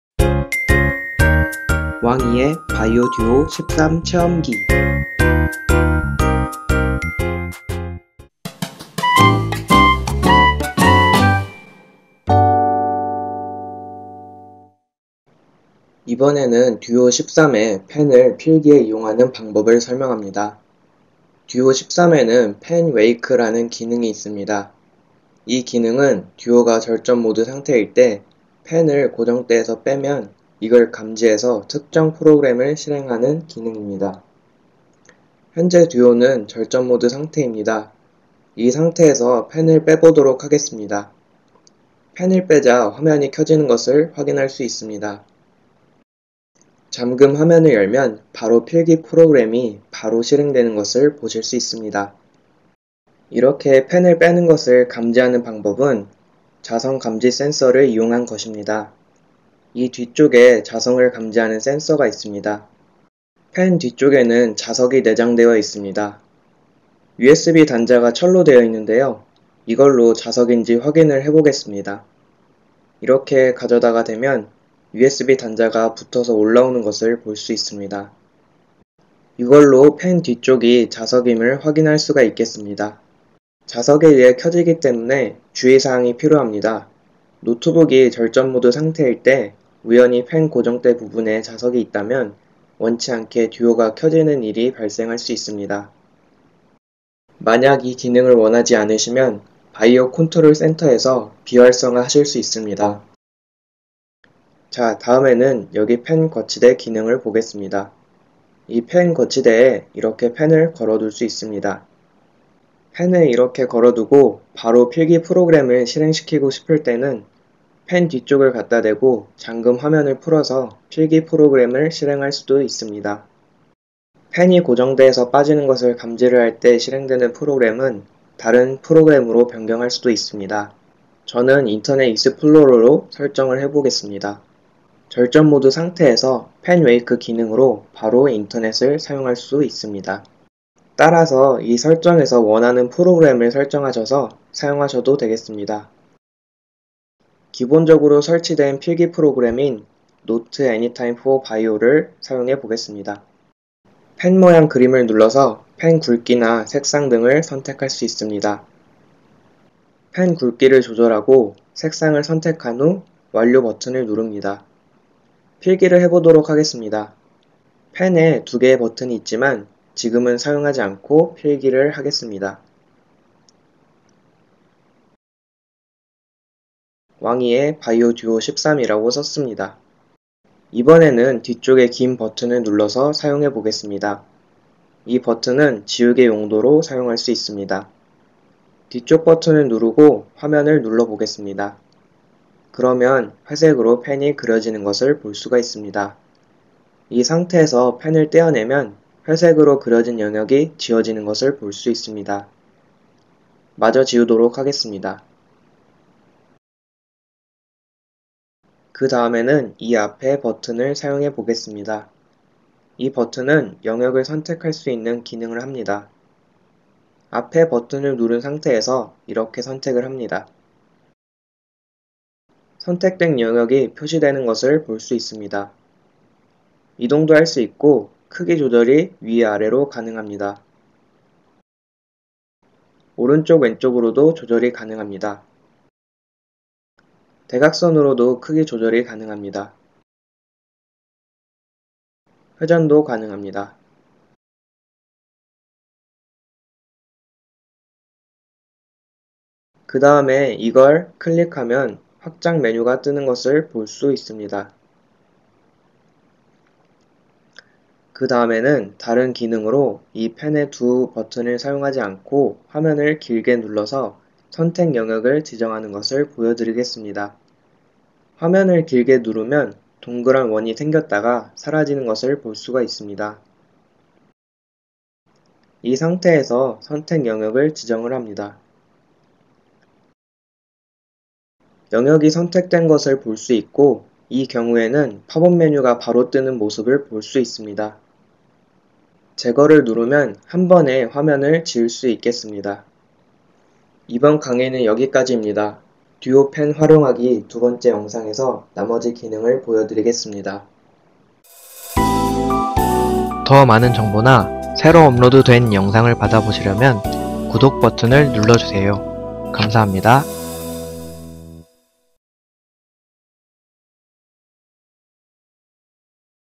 04 VAIO Duo 13(바이오 듀오) 펜 활용 하기 1 (Using a pen 1)
Supports English subtitles.

이 동영상이 맘에 드셨다면, 좋아요 (乃)  버튼을 눌러주세요!
この動画を気に入ると、いいね (乃) ボタンを押してください！
If you like this video, please click the like (乃) button !

이번 영상은 바이오 듀오 13의 펜을 필기에 활용하는 방법을 설명 합니다.

다음의 내용을 설명 합니다.
•Fan wake 기능
•펜이 빠지는 것을 감지하는 방법
•펜 거치대 기능
•절전 모드 상태일때 주의사항
•Fam wake 기능으로 특정 프로그램을 실행하는 설정
•Note Anytime for VAIO 필기 프로그램 사용 방법
•Note Anytime for VAIO 필기 프로그램에서 듀오 펜의 버튼을 사용하는 방법